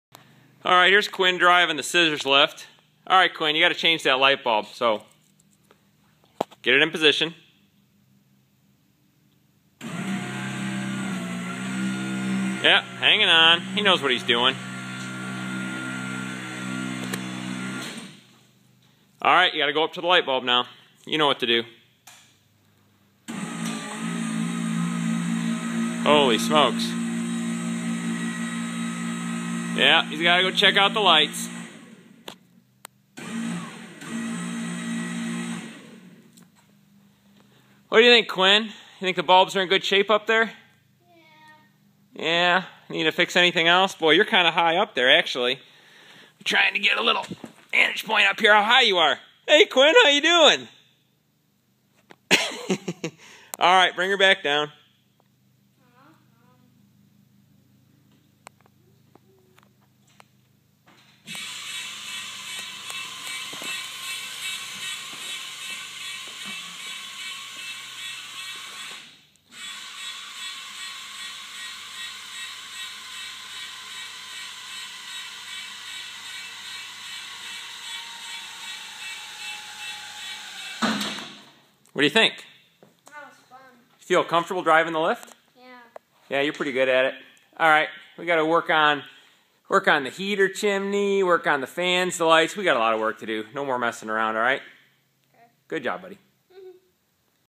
Quinn drives the scissor lift
Here's my 7 year old learning how to drive a lift as we work in our hangar.